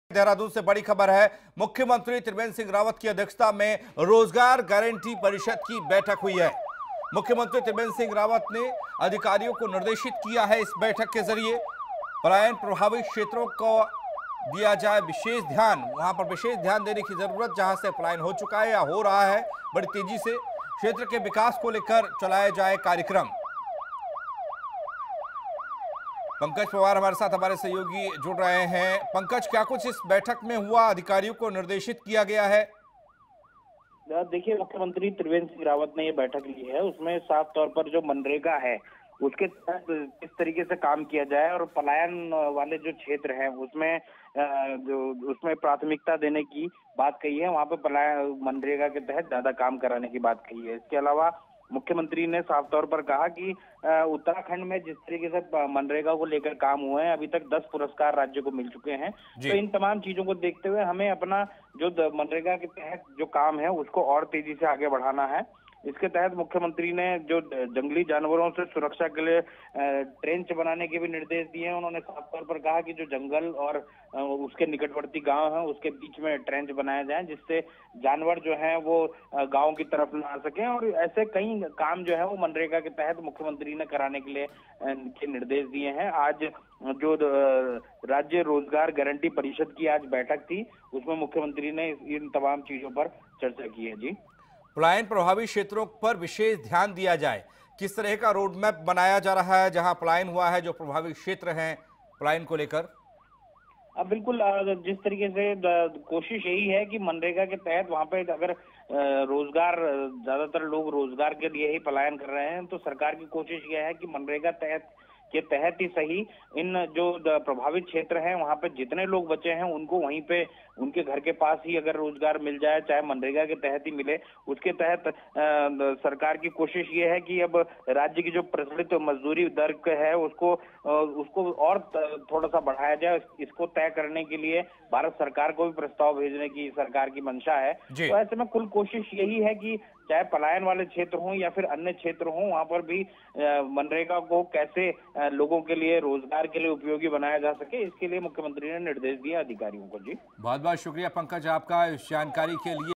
देहरादून: रोजगार गारंटी परिषद् की बैठक, सीएम त्रिवेंद्र ने अधिकारियों को दिए निर्देश R9TV
देहरादून: सीएम त्रिवेंद्र की अध्यक्षता में रोजगार गारंटी परिषद् की बैठक
सीएम त्रिवेंद्र ने अधिकारियों को दिए निर्देश
'पलायन प्रभावित क्षेत्रों का दिया जाए विशेष ध्यान'
'क्षेत्र के विकास को लेकर चलाए जाएं कार्यक्रम'
WATCH  -  R9 TV
TATA SKY - 586
AIRTEL - 352